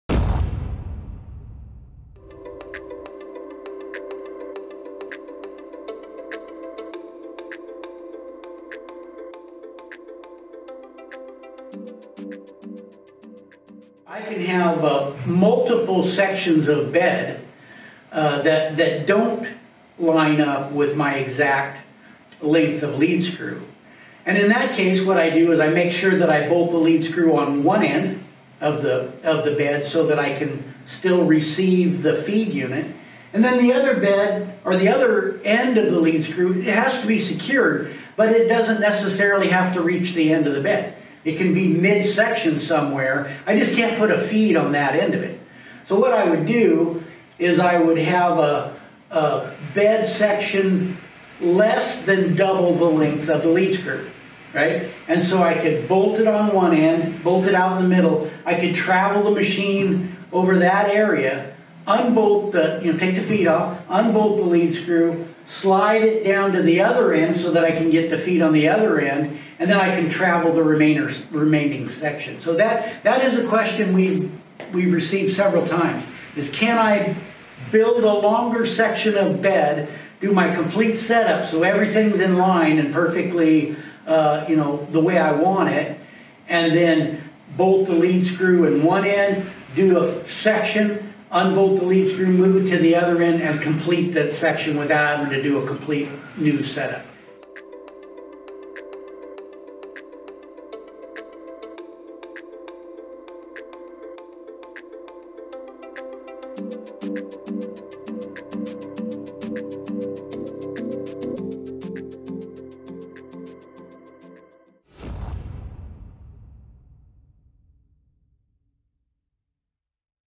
CLIMAX Milling Machine: Can you extend the length of travel without having a whole new setup?
Can you extend the length of travel without having a whole new setup? See what Jim Miller has to say in a clip from our Milling Machine LIVE forum.